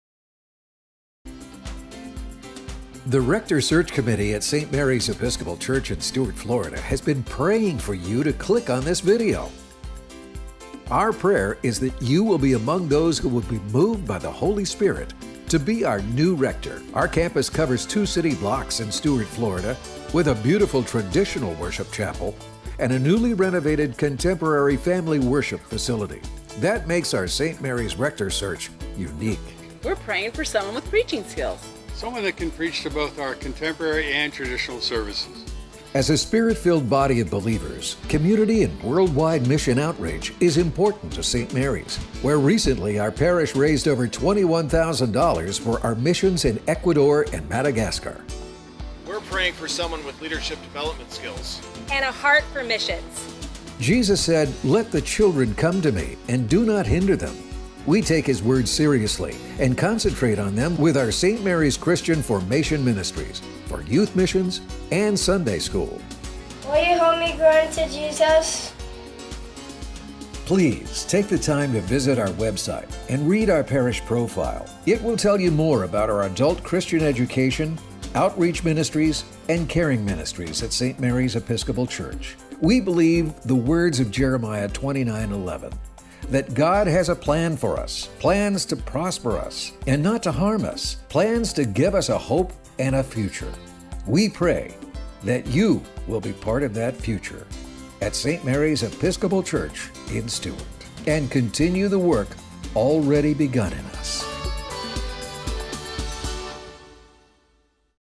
Saint Mary's Episcopal Church - Rector Search
Spread the word as St. Mary's Episcopal Church In Stuart FL look for our next Rector.